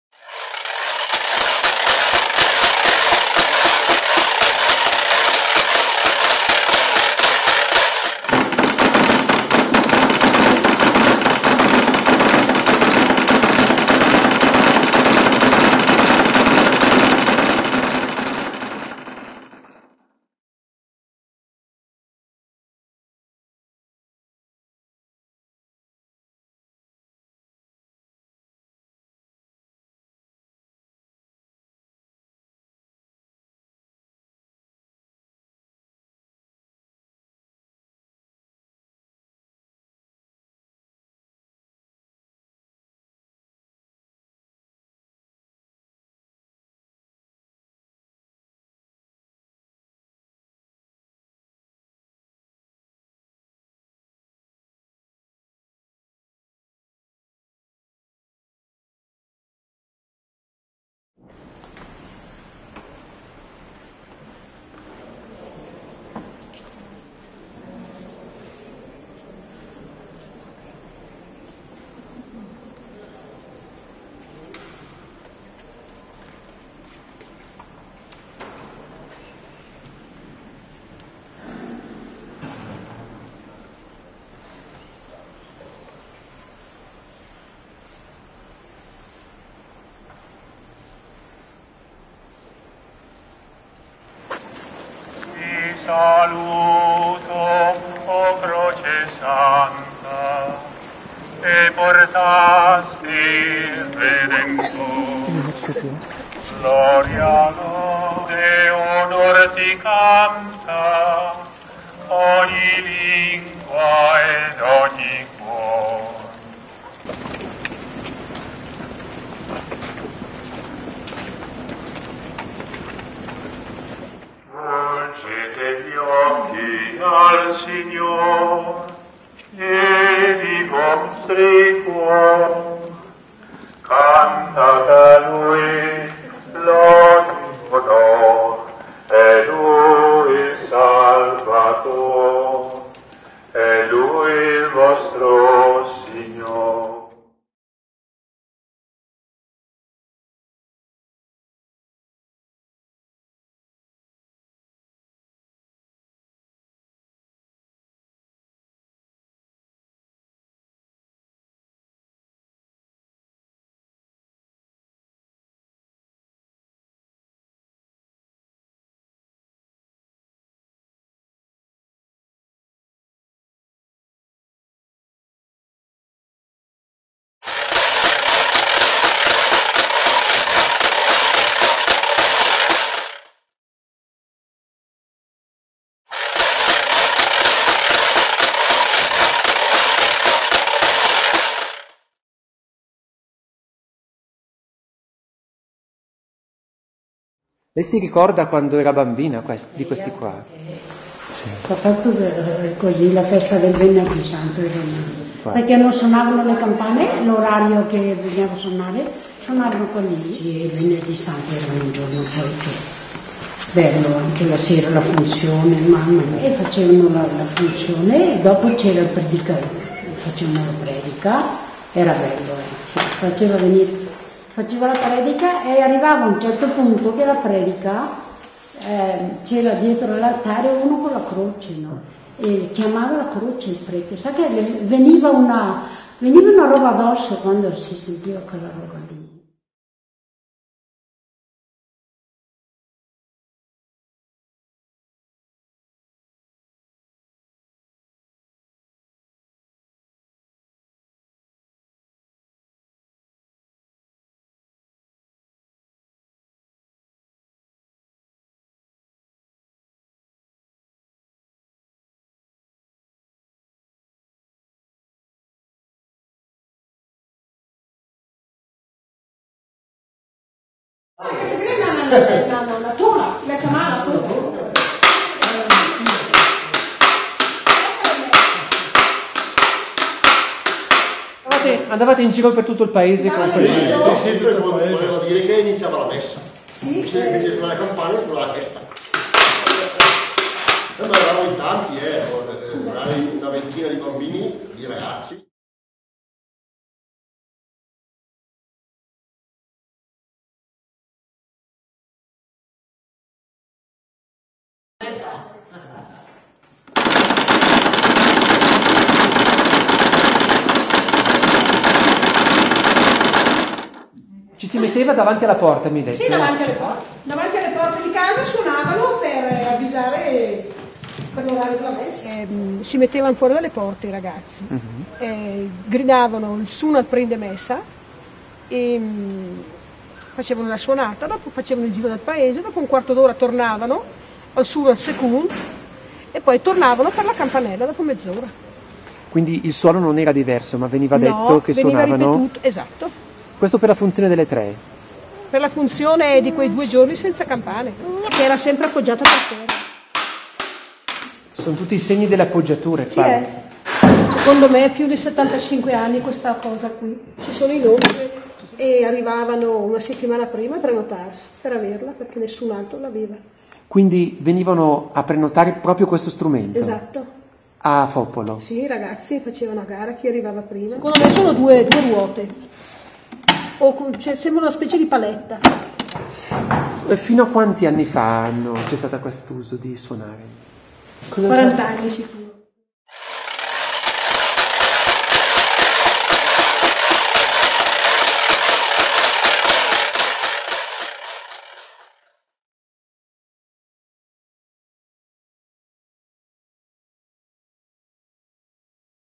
Tradizioni del Venerdì Santo in Valle Brembana
Documentario sugli strumenti di richiamo religioso del Venerdì Santo in provincia di Bergamo. Federazione Campanari Bergamaschi. Vedi articolo collegato